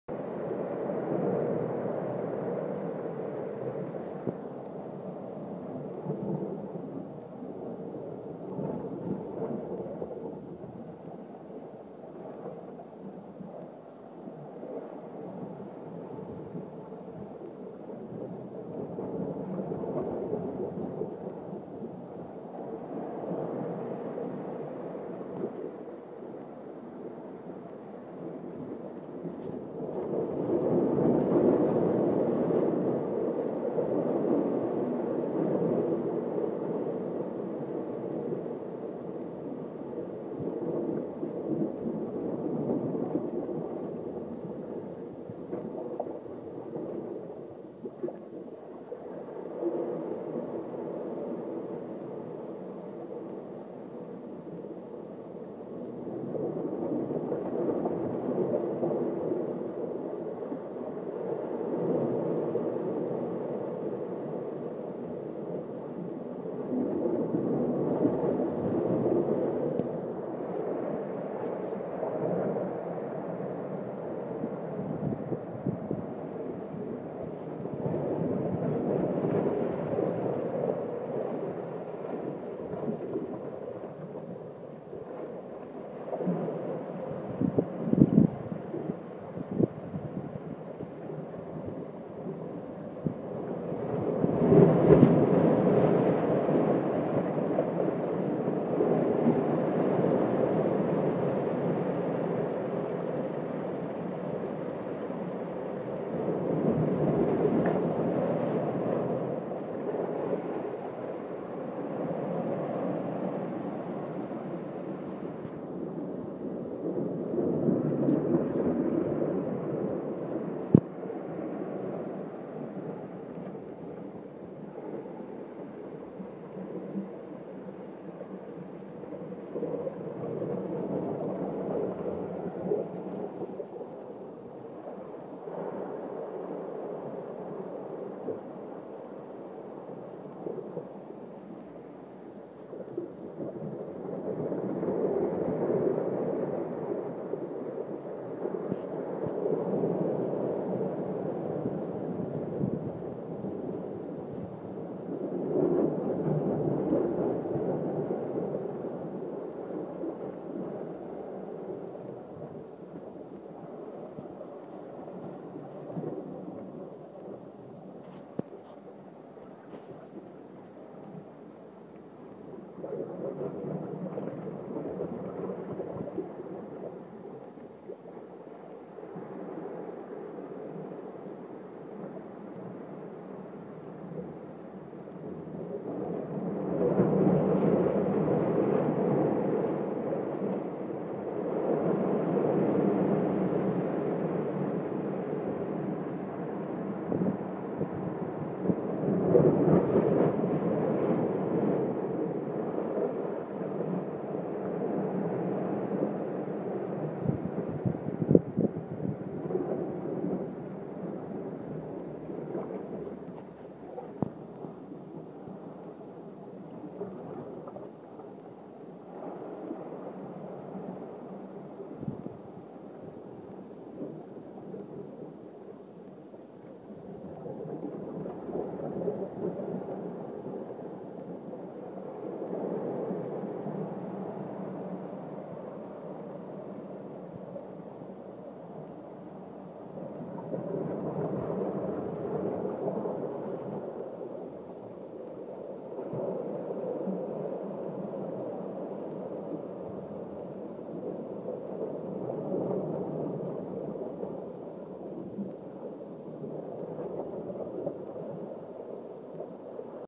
Glug
Surf gurgles into deep pools.